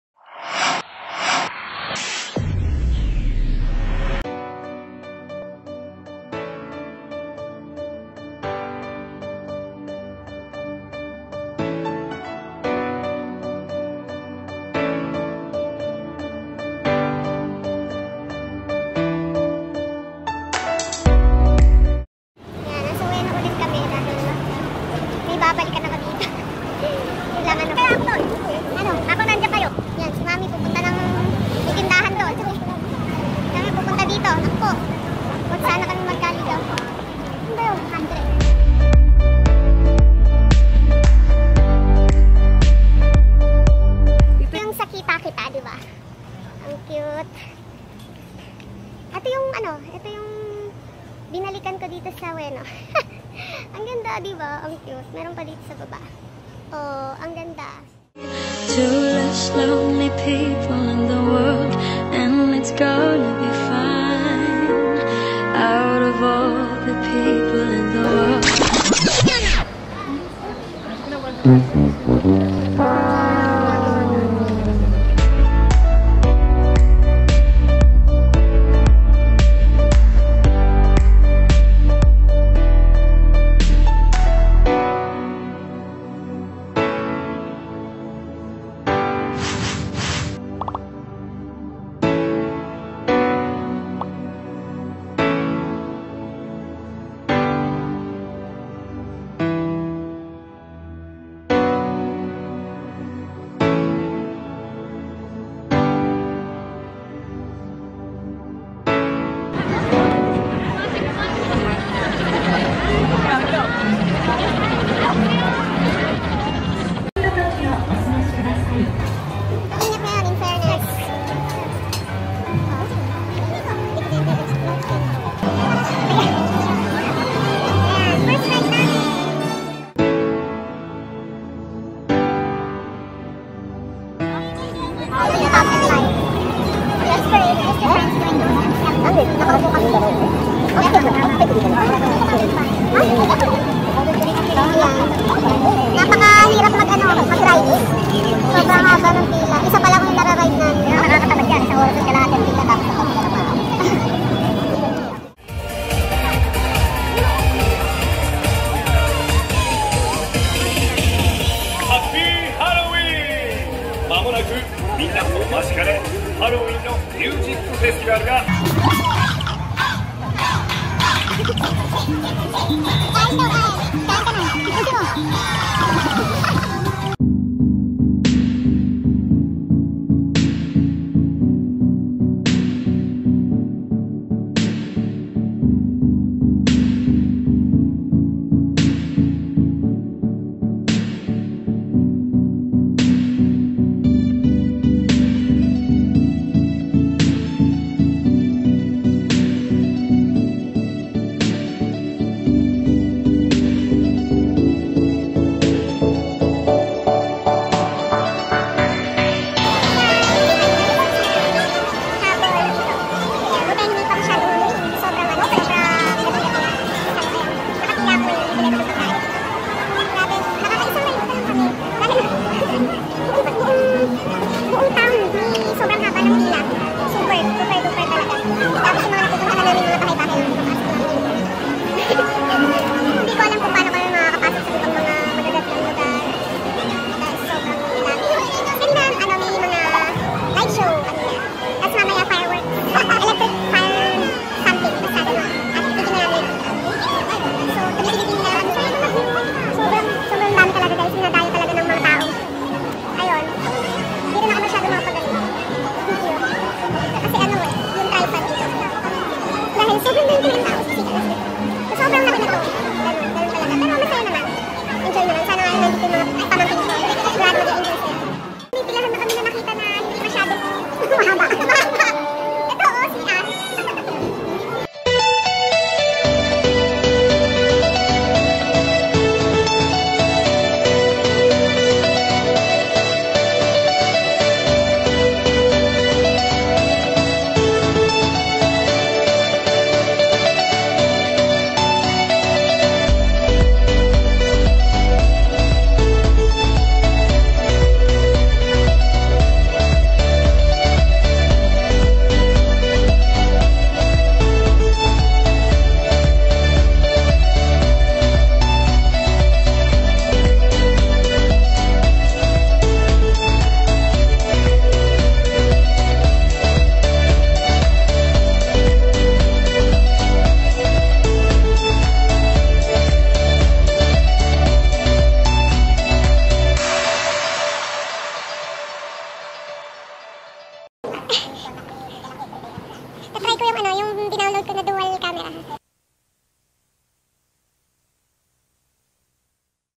JAPAN DAY 5 - TOKYO DISNEYLAND!
Beautiful place to visit. Experience a Disney feels here in Tokyo Disneyland! What a wonderful experience it is. :)